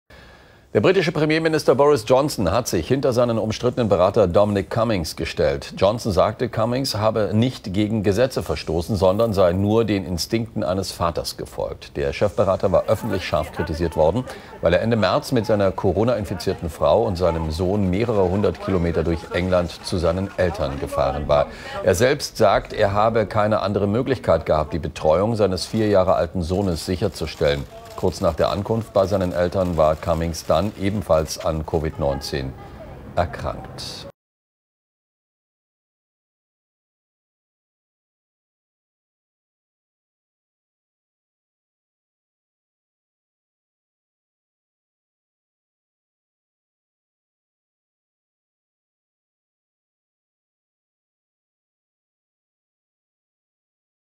CORONA IN GROSSBRITANNIEN: Johnson stellt sich hinter seinen Chefberater Cummings
Der britische Premierminister Boris Johnson hat mit seiner Rückendeckung für Chefberater Dominic Cummings einen Sturm der Entrüstung ausgelöst und gerät nun selbst zunehmend unter Druck. Mehr als ein Dutzend Parlamentarier aus Johnsons konservativer Partei, die Opposition, Geistliche und Ärzte forderten den Rücktritt Cummings', weil er gegen Ausgangsbeschränkungen verstoßen haben soll.

Cummings wird vorgeworfen, mit einer Reise von London zu Familienangehörigen ins rund 430 Kilometer entfernte Durham Ende März gegen Ausgangsbeschränkungen in der Corona-Krise verstoßen zu haben. Der Wahlkampfstratege gilt als eine Art Strippenzieher in der Regierung, der hochintelligent und zugleich unberechenbar ist.

Cummings gab als Grund für seine Reise an, er habe keine andere Möglichkeit gehabt, die Betreuung seines vier Jahre alten Sohnes sicherzustellen. Er habe für die Betreuung seines Kindes sorgen wollen, weil seine Frau an Covid-19 erkrankt gewesen sei und er selbst auch mit einer Ansteckung habe rechnen müssen.

Cummings sei «den Instinkten eines jeden Vaters gefolgt», sagte Johnson am Sonntag. Dafür könne er ihn nicht an den Pranger stellen. Nach den Worten Johnsons hat sein Chefberater «in jeder Hinsicht verantwortlich, legal und mit Integrität» gehandelt.